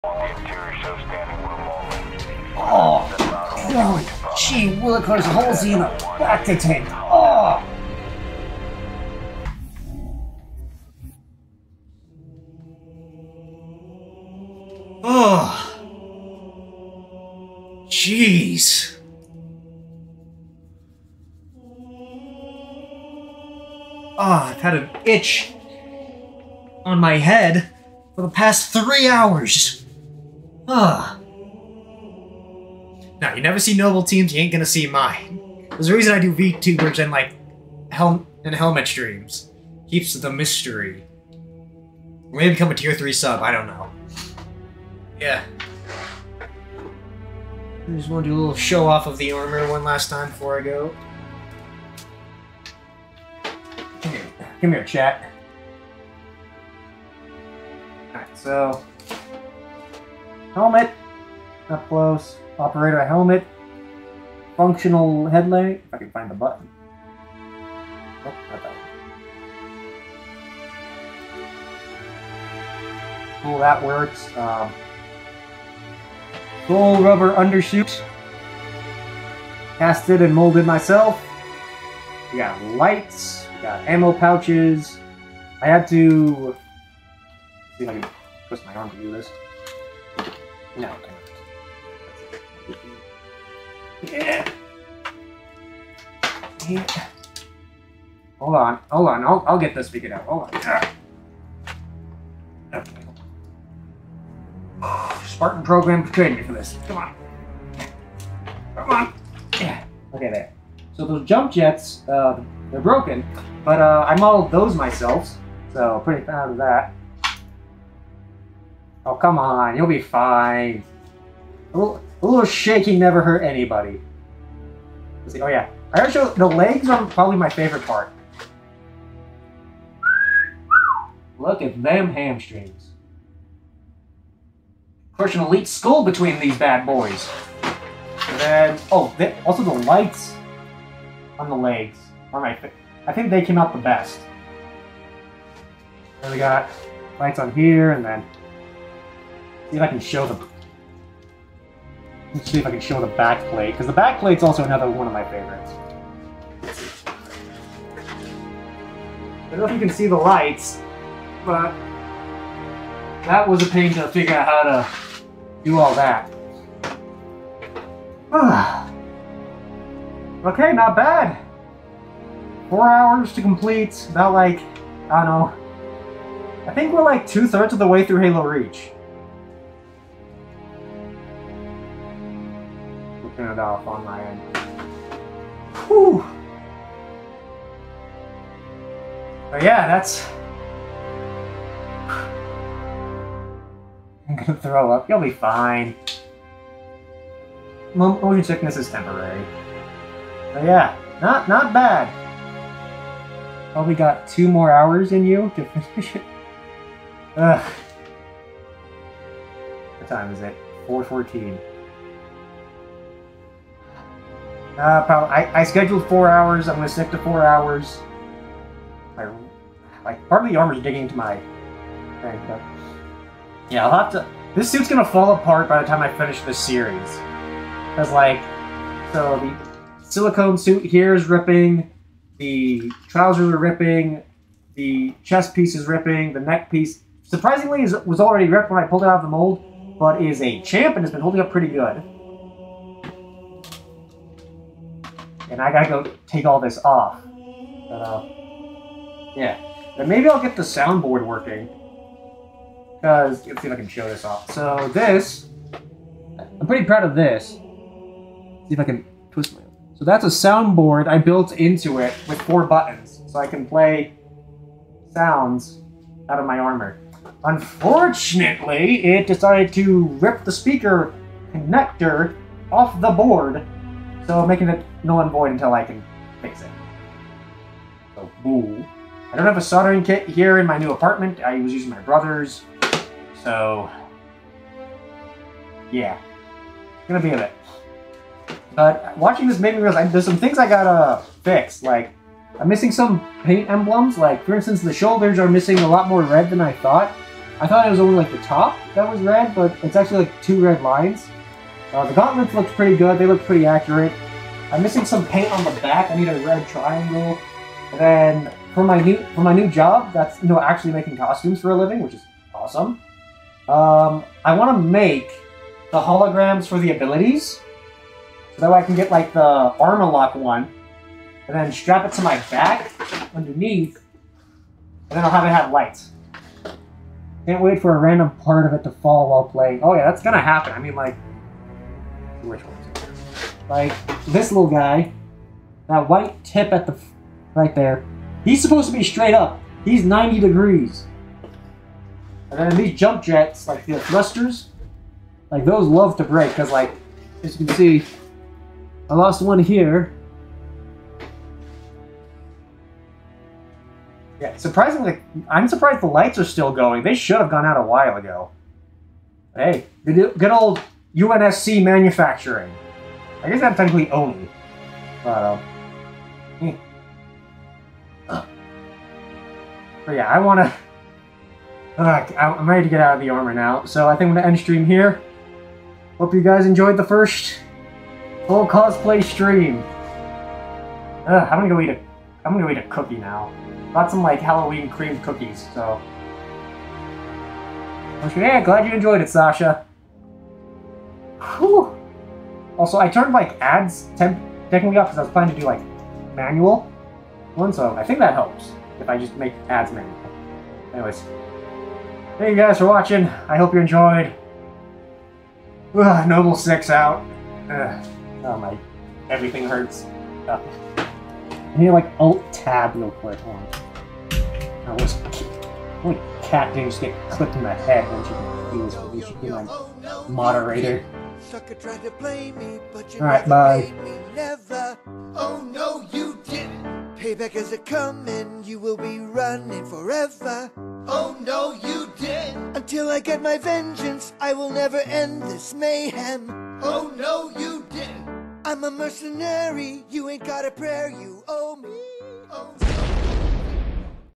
Armor Showcase after stream
One last show off of the armor before I ended stream